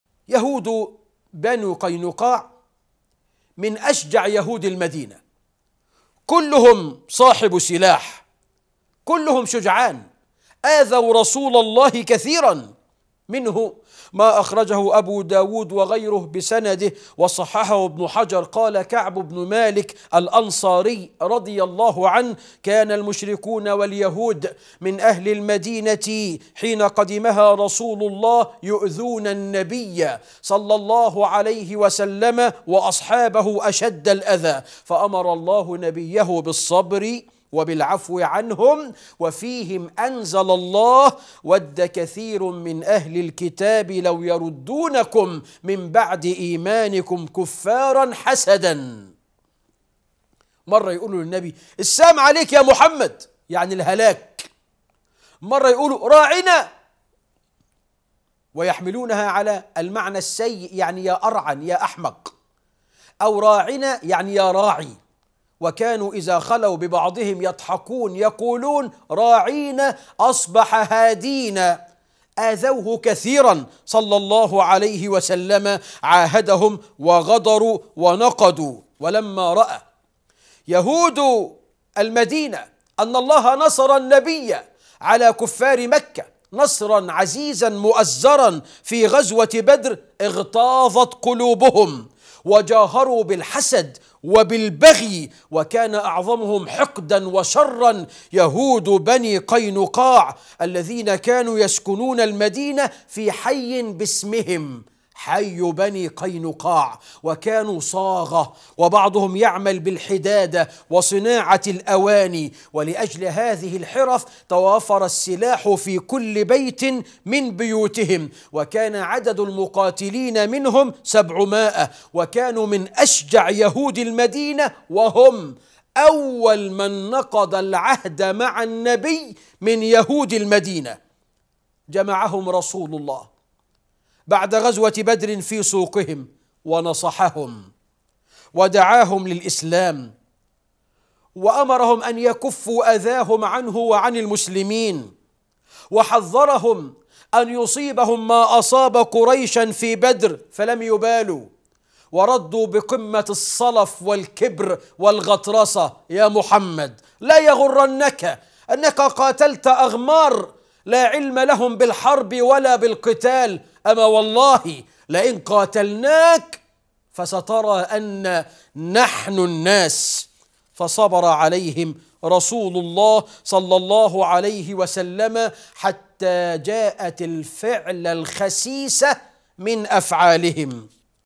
بنو قينقاع أشجع الشجعان !! الشيخ مسعد انور أنور
من لقاء غزوة بني قينقاع دروس وعبر || آل البيت والصحابة || فضيلة الشيخ مسعد أنور
|| آل البيت و الصحابة ||